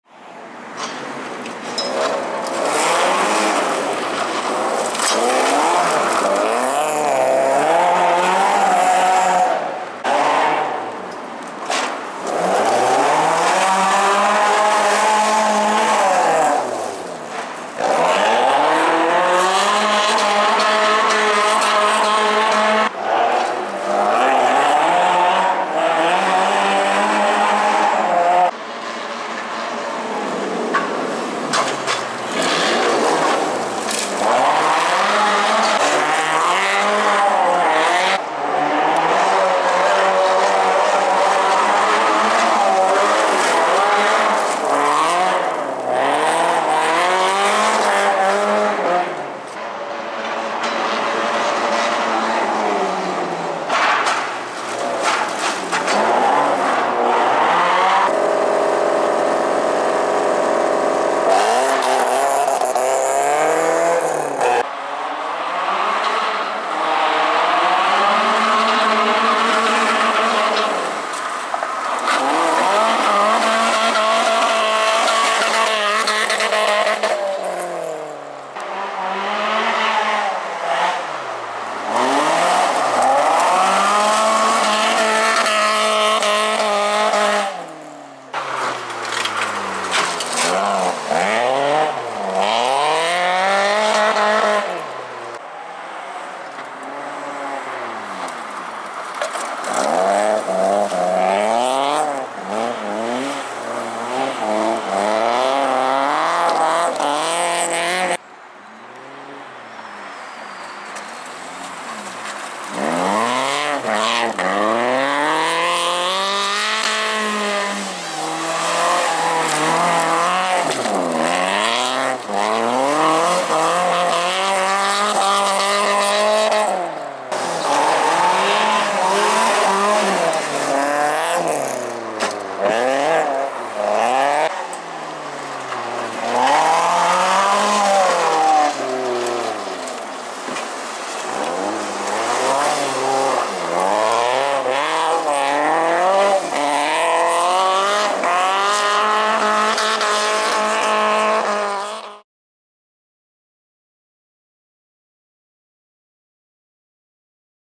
BMW E36 Compact (Marcin Szwejk) | 2. Serwal Rally 2024
2. Serwal Rally Day (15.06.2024). Teren piaskarni Serwal pod Mostem Południowym w Warszawie.
2 runda Samochodowych Mistrzostw Pucharu AK Centrum.

BMW E36 318ti Compact - Marcin Szwejk

FULL HD 60FPS nagrane kamerą Sony HDR-CX405.